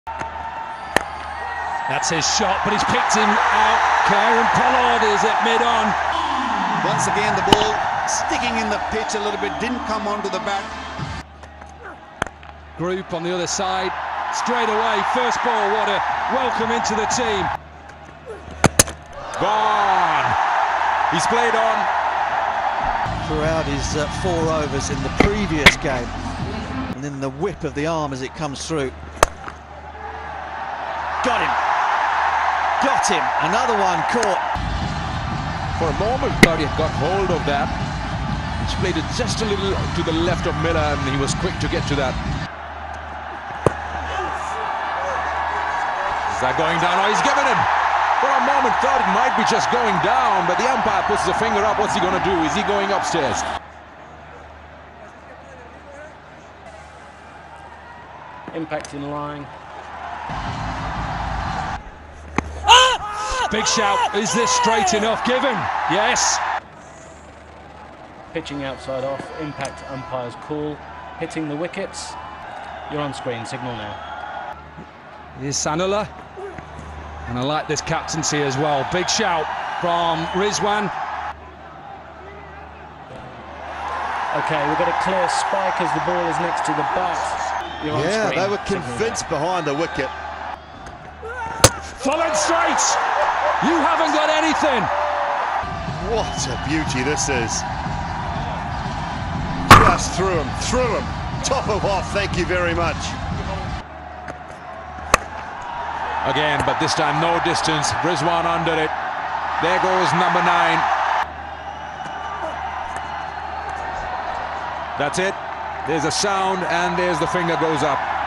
Outstanding Bowling By Multan | Quetta All FOW | Multan vs Quetta | Match 3 | HBL PSL 8 | MI2A
Outstanding Bowling By Multan | Quetta All Fall of Wickets | Multan Sultans vs Quetta Gladiators | Match 3 | HBL PSL 8 | MI2A